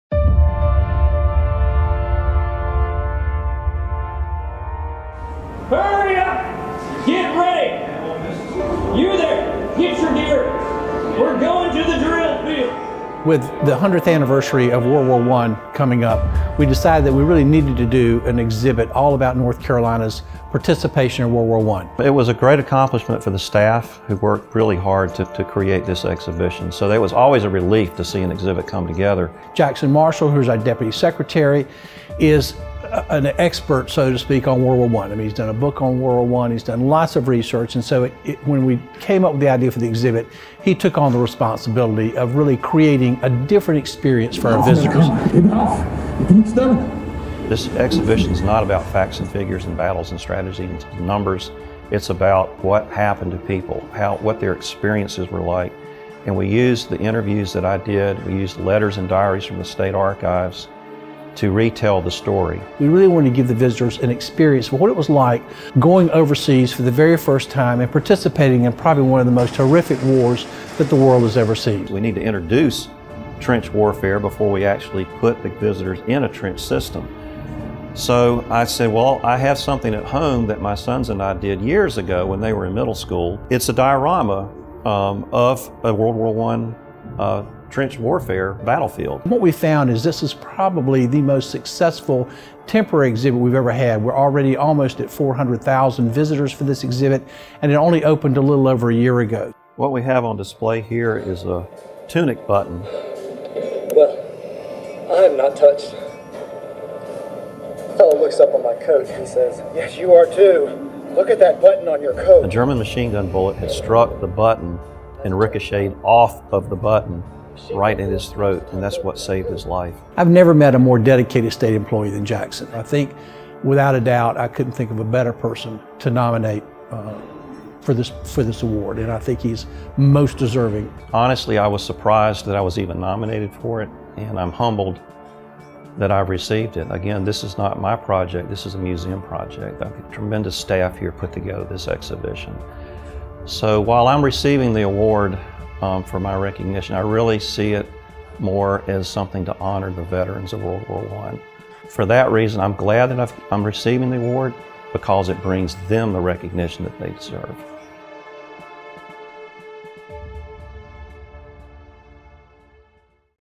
Deputy Director Jackson Marshall Receives Governor's Award for Excellence
On October 23, 2018, Deputy Director, Jackson Marshall, received the Governor's Award for Excellence in "Outstanding State Government Service!"

When Jackson was a young man, he made a promise to World War I veterans to never let their stories be forgotten. His life's work of collecting stories and artifacts from the war culminated in the creation of our award-winning exhibit, "North Carolina & World War I!" This exhibit has set a new museum record with over 470,000 visitors!